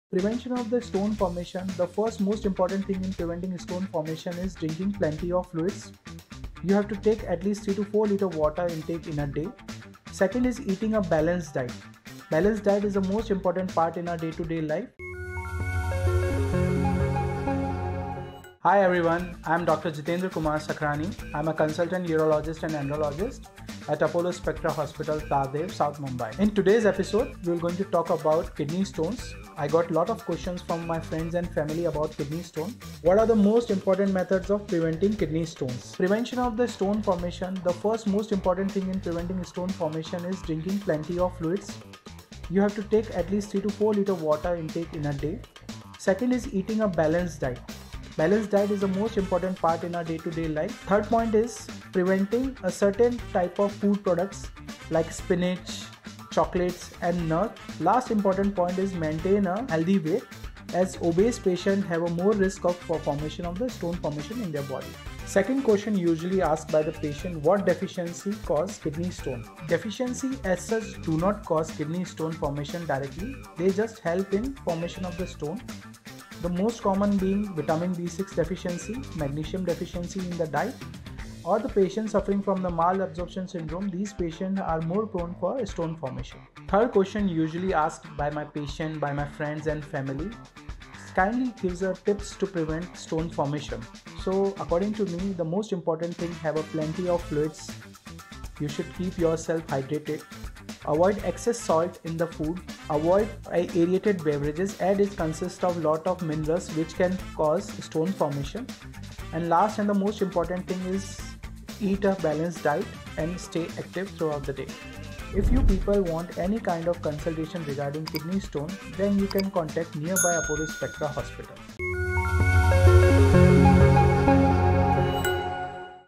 Kidney Stones: Causes and Prevention Tips by Dr. Jitendra Sakhrani, Apollo Spectra
Kidney stones are hard deposits of minerals and acid salts that stick together in concentrated urine. They can be painful when passing through the urinary tract but usually don't cause permanent damage. Watch Dr Jitendra Sakhrani, a Urologist from Apollo Spectra Hospitals, discuss the Causes and Prevention Tips for Kidney Stones.

Annotations:

0:36 - What are the most important things to do to prevent kidney stones?
1:10 - Can vitamin deficiency cause kidney stones?
1:34 - What are the tips to prevent kidney stones?